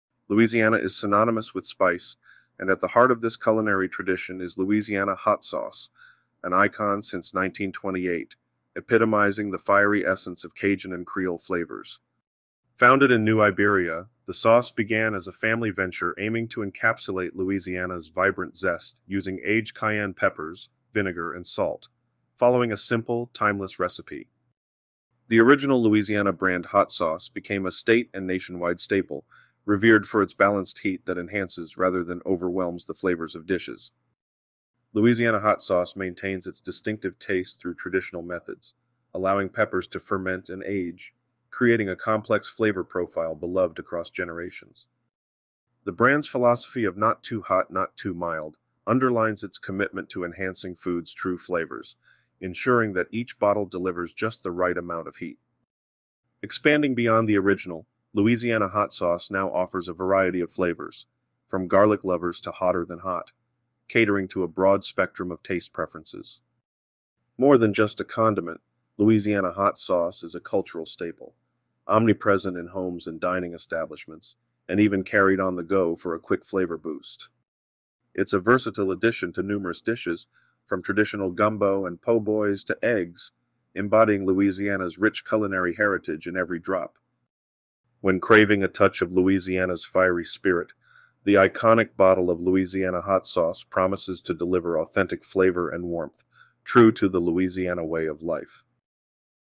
The Story of Louisiana Hot Sauce - An Icon Since 1928
Louisiana is famous for its food, and nothing says Louisiana flavor like a splash of the Original Louisiana Hot Sauce! In this video, we're diving into the history of this iconic hot sauce company. We'll explore how it's made, where the tradition came from, and of course, taste some of their hottest flavors. Get ready to add some spice (and a bit of history) to your life!

Louisiana Hot Sauce
Louisiana food
hot sauce history
hot sauce review
Cajun food
Creole food
spicy food challenge
New Iberia